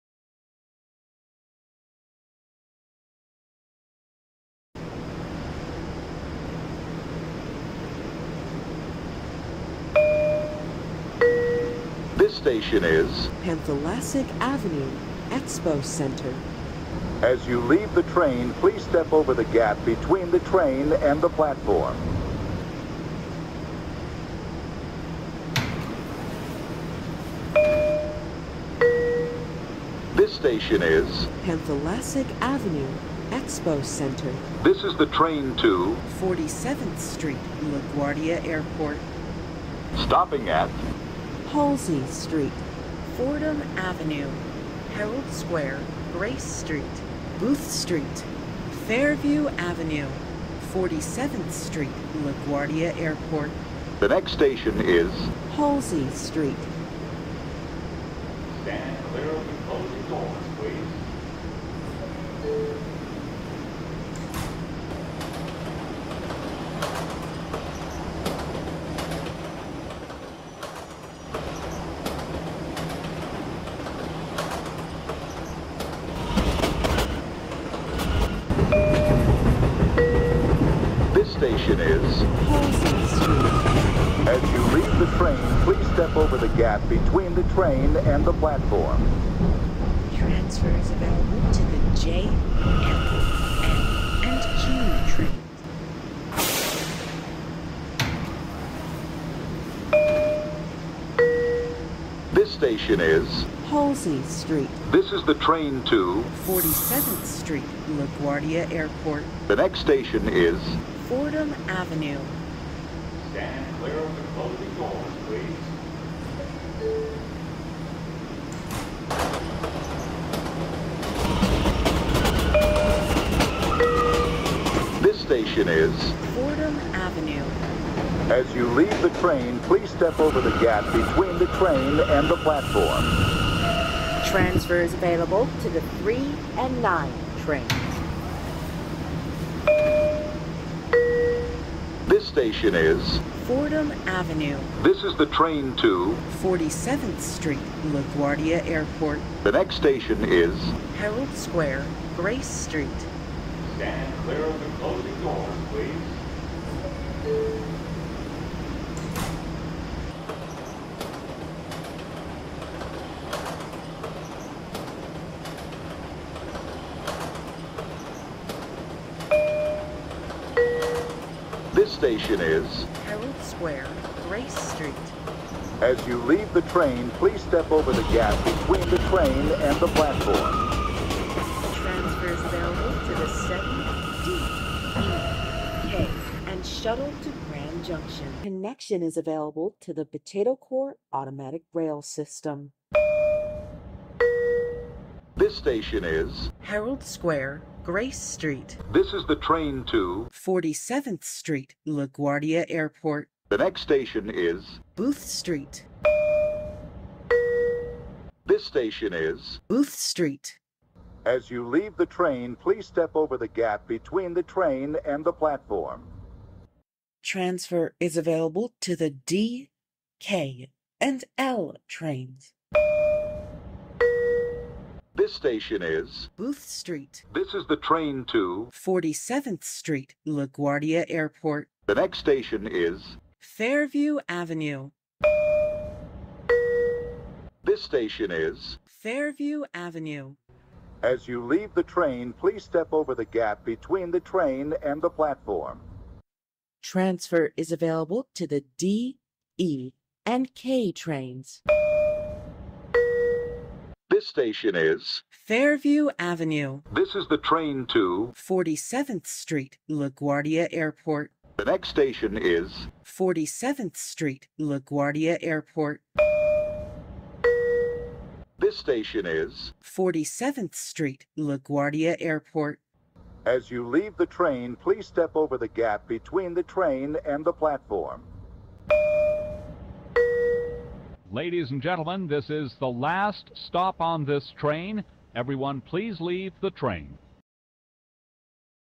R179 April Fools B Train Automated Annoucements to 47 Laguardia
Credit to: TomyTabby 2
For annoucements from Herald Sqaure (Mine got corruptede from Herald Sq :p)